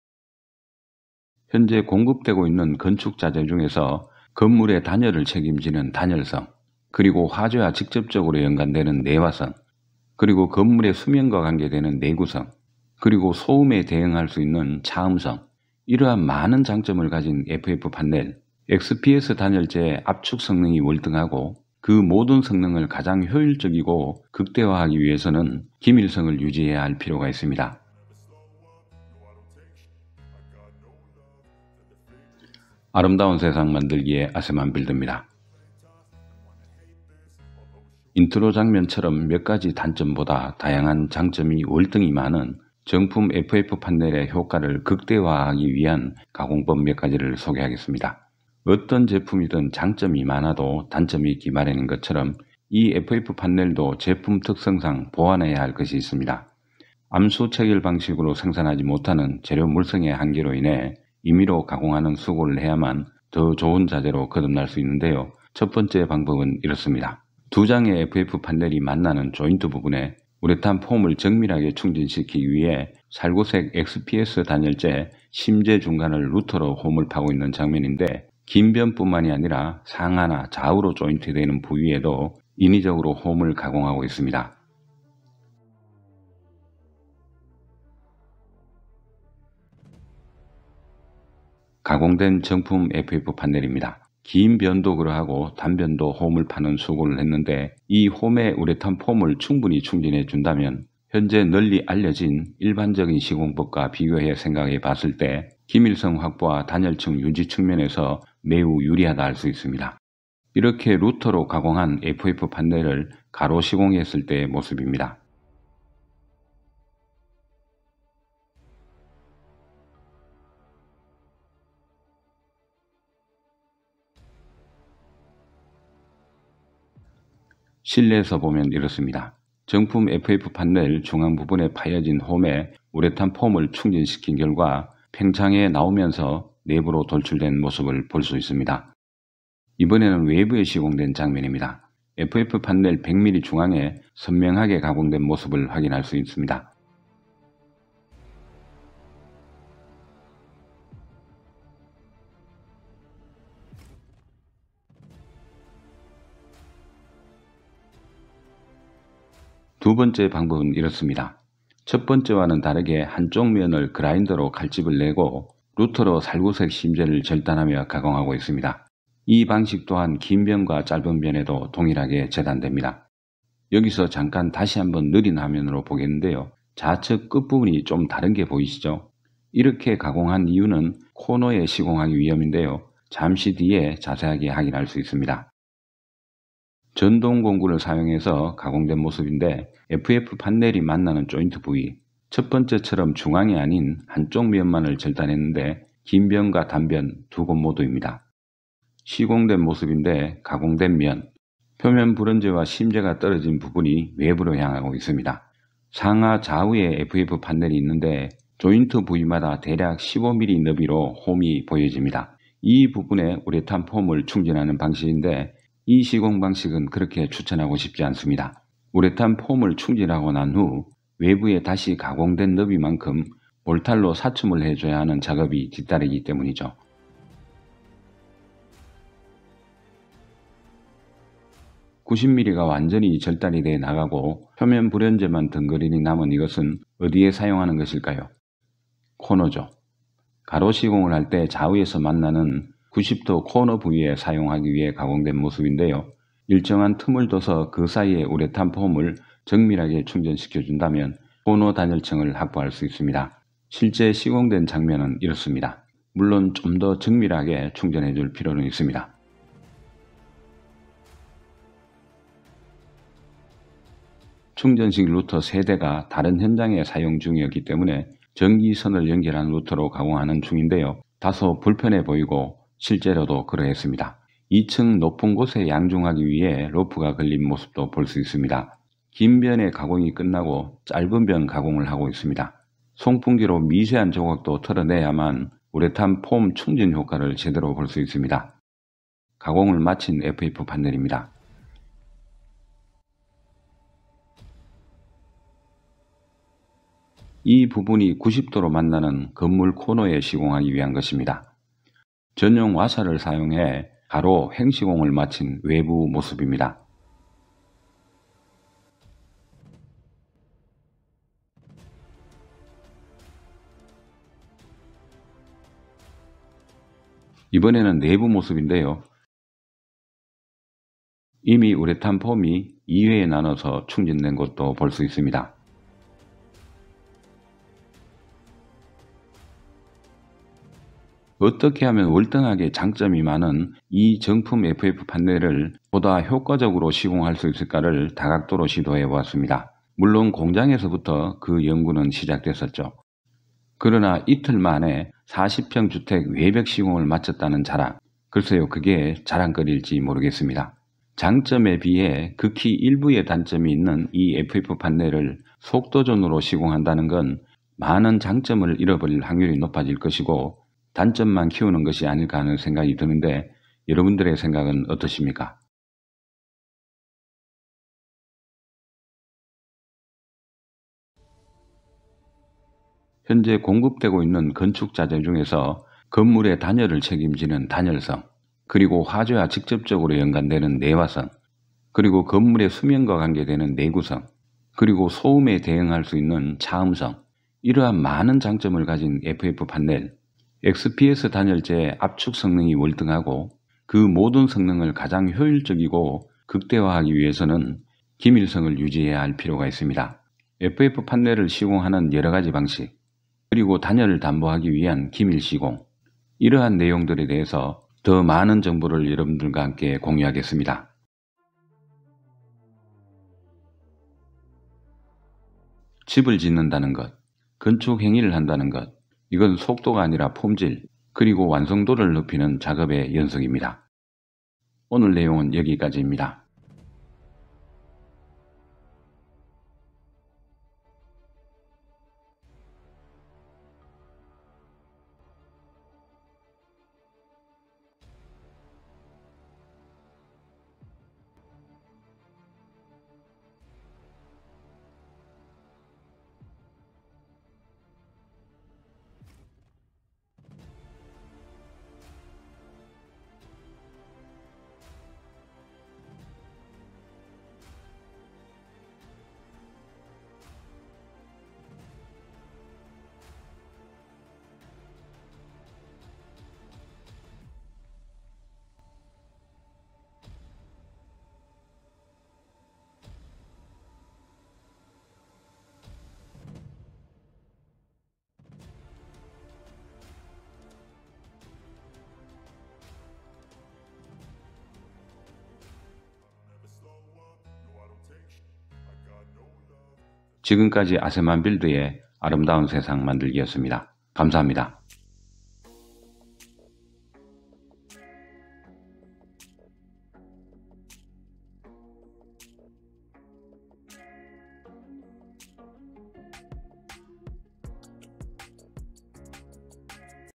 FF판넬시공 정석 단열 차음 내화성 최고,FF판넬,FF판넬시공정석,ff판넬 정석시공법,ff판넬반축공사비,경량철골주택,ff판넬주택,ff판넬시공법,FF판넬시공과정,ff판넬정석시공
FF판넬 재료물성을 파악하지 못하고 시공한다면 어떻게 될까요?
월등하고 우수한 자재인 경량 콘크리트 FF판넬을
제대로 시공하기 위해서는 재료에 대해 정확히 알아야 합니다.

단열성능, 내화성능, 차음성능, 압축성능, 내구성능 등의 다양한 기능성을 가진
이 FF판넬의 효과적이고 최적화된 시공방법과 가공방법 3가지를 소개합니다.

부디 많은 분들께 도움되는 정보이길 희망합니다.
감사합니다^^

-아름다운 세상 만들기 가족일동-